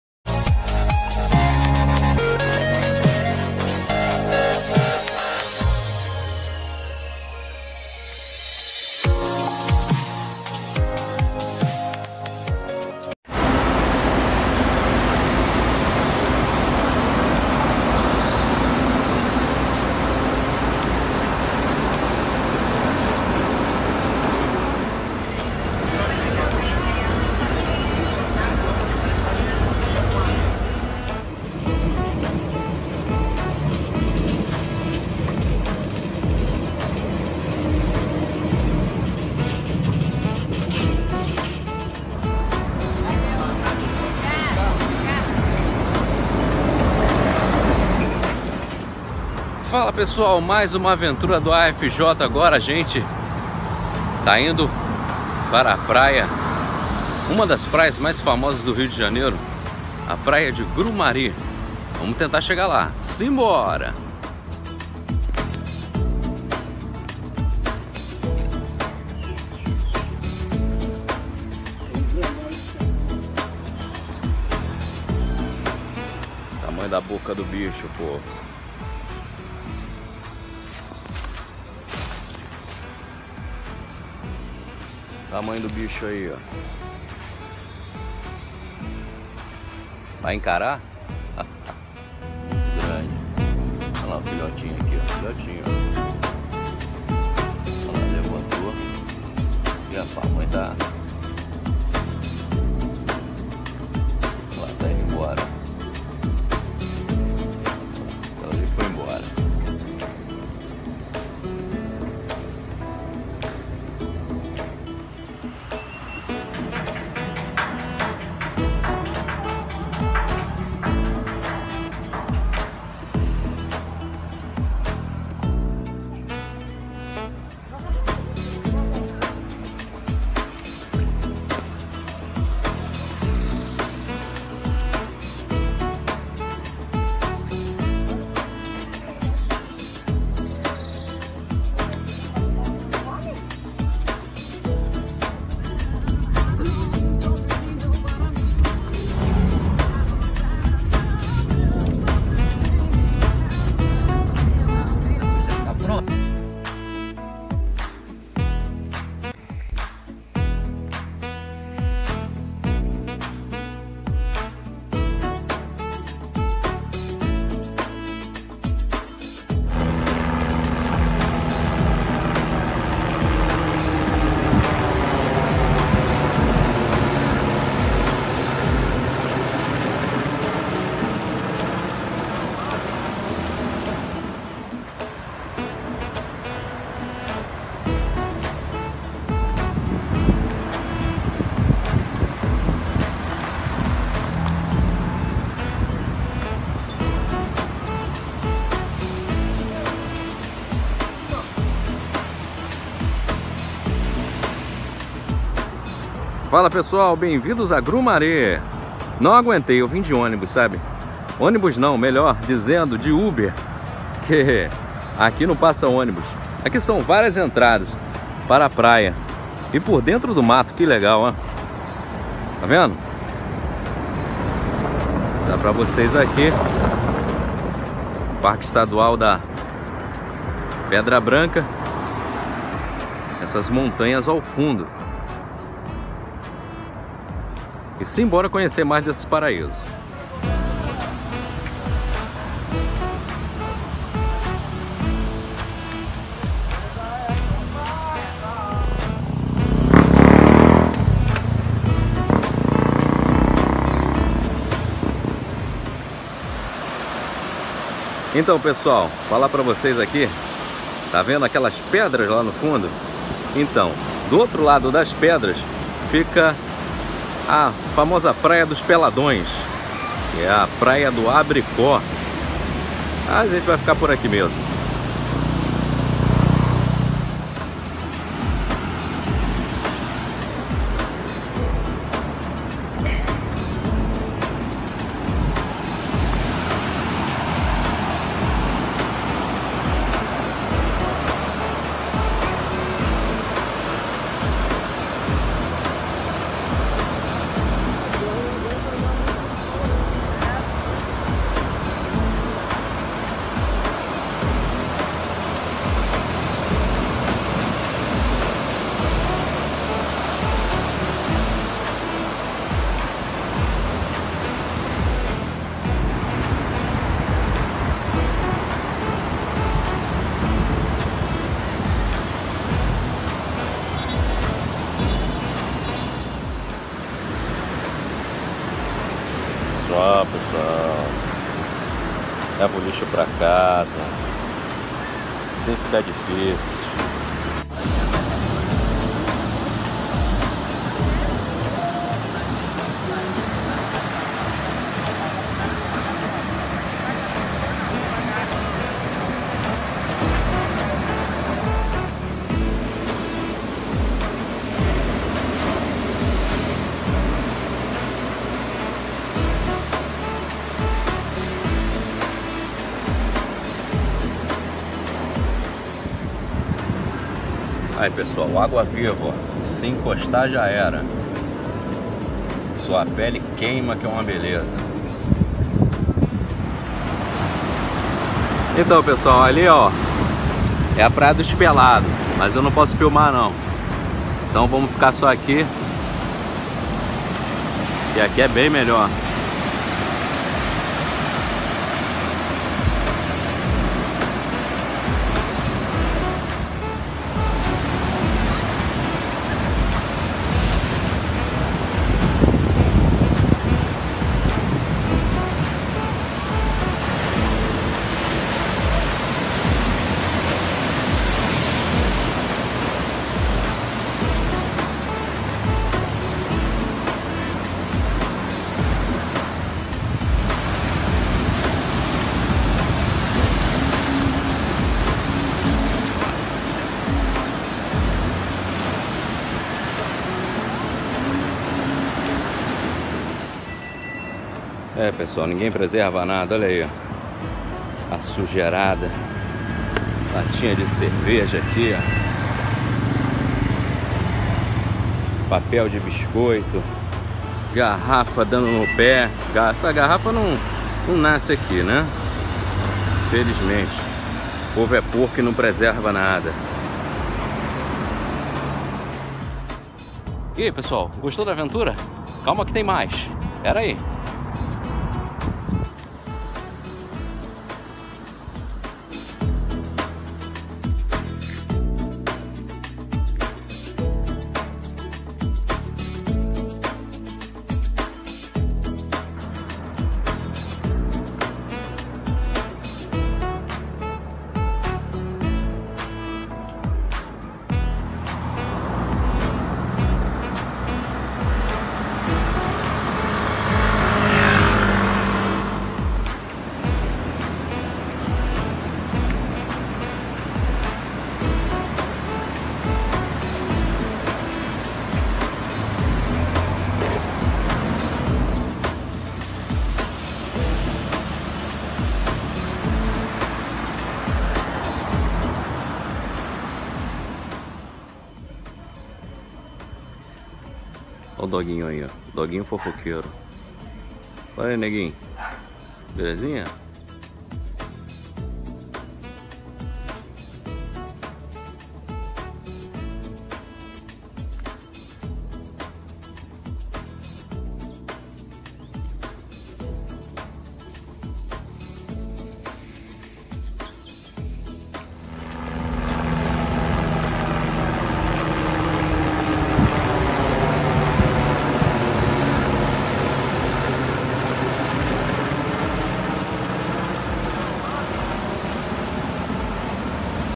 PRAIA DE GRUMARI - RIO DE JANEIRO - Como chegar
Uma das praias mais bonitas da cidade do Rio de Janeiro, conhecida também como praia da reserva da zona oeste da cidade.

✅ Como chegar: Solte na estação BRT Recreio Shopping e vá até a avenida atlântica, de lá, você pode pegar o Uber até o local, que é bem barato.